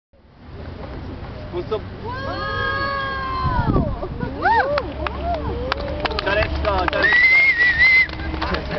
Acrobatic Gymnastic in South Korea
A little acrobatic line at Chungcheong, near a traditional pagoda! What a beautiful anthropological respect!!! Thousand thanks to the Korea Foundation for this unforgetable trip!